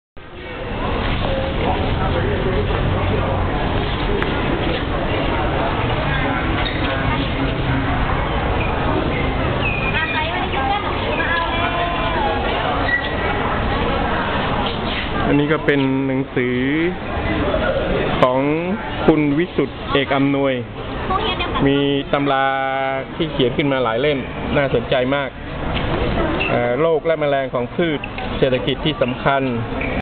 Thailand, RSTDC : Science Fair at Chiangmai University37
There was Science Fair at Faculty of Science, Chiangmai University during 18-20 August 2009, on the 19 night there was big strom that flow down the exhibition broads, mostly of exhibitions were closed down, only three exhibitors stiil reorganized and went on exhibition